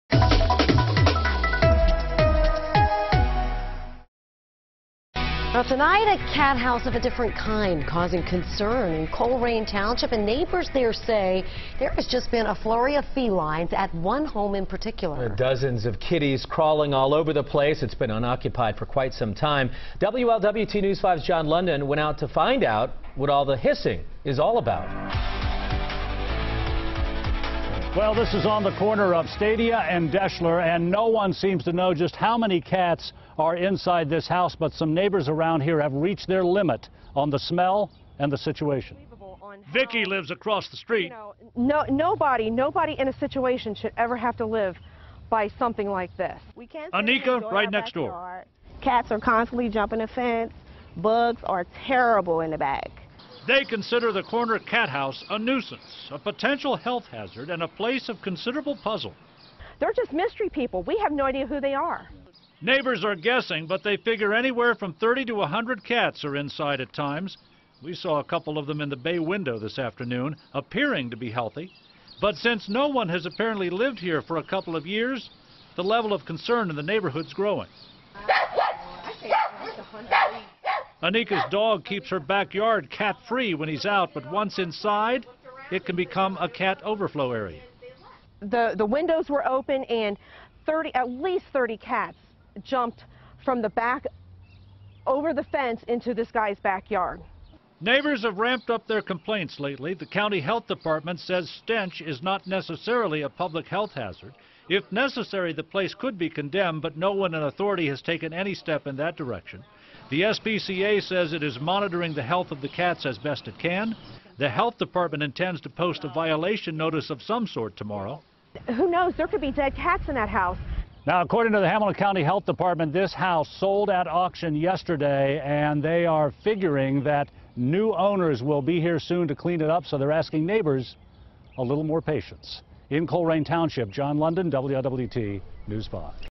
Neighbors demand action on house overrun with cats
Vicki Harris lives across the street from a place that she says is overrun with cats and attracting flies, and hasn't been lived in for a couple of years.